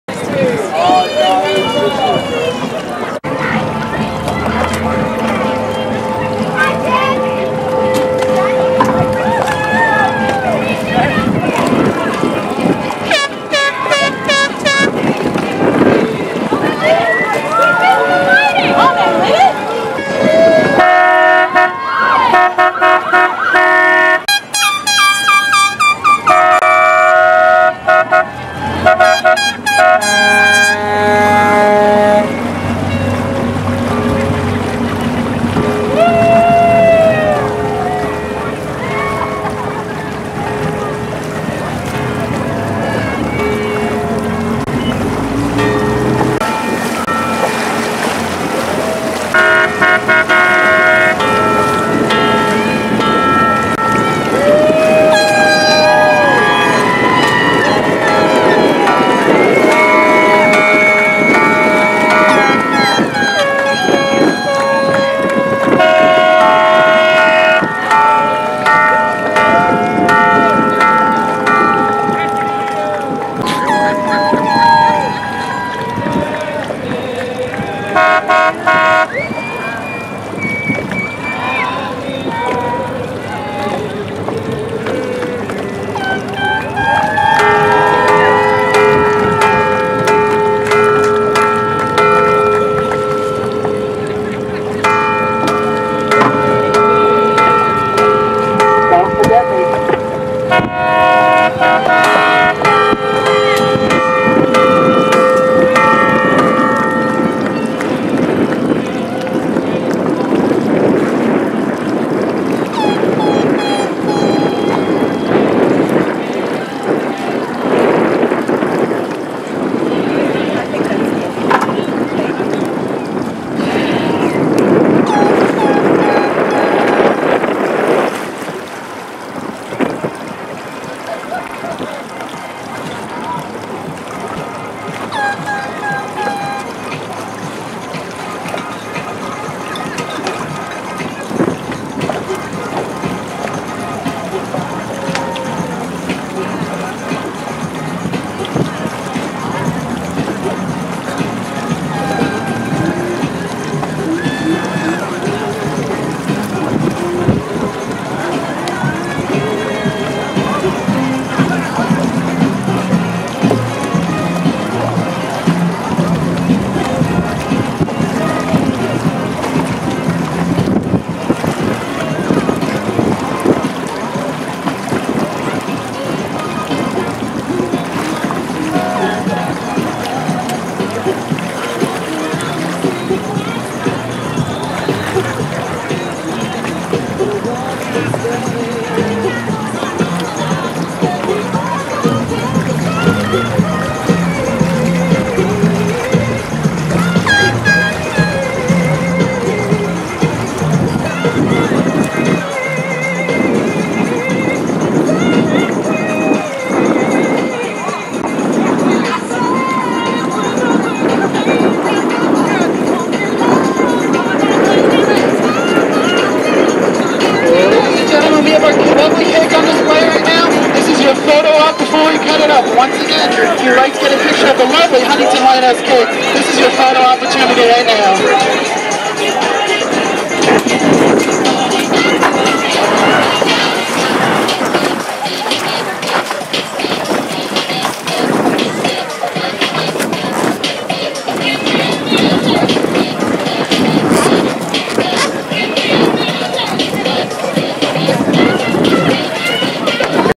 Huntington Lighthouse Centennial Celebration
Check out this 360 pass of the Hungtington Harbor Lighthouse as the historic struture turns 100 years old.